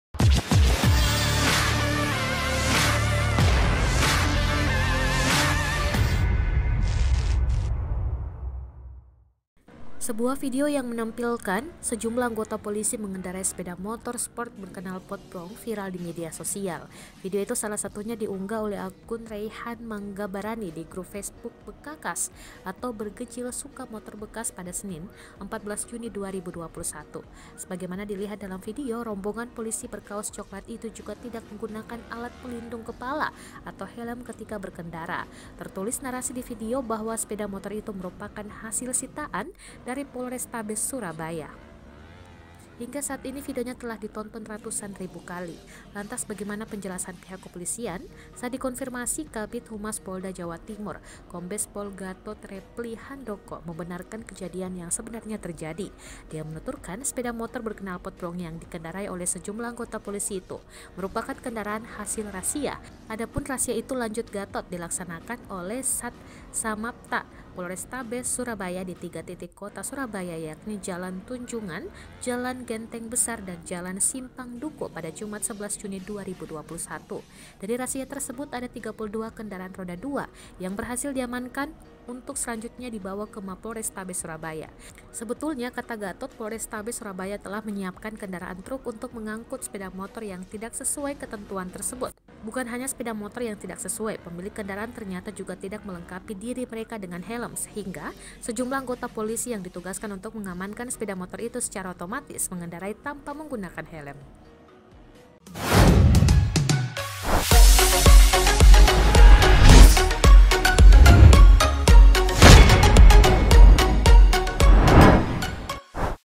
Video Rombongan Polisi Kendarai Motor Sport Berknalpot Brong dan Tak Memakai Helm
Video itu salah satunya diunggah oleh akun Reyhan Manggabarani di grup Facebook Bekakas (Bergejil Suka Motor Bekas), Senin (14/6/2021).

(TRIBUN-TIMUR.COM)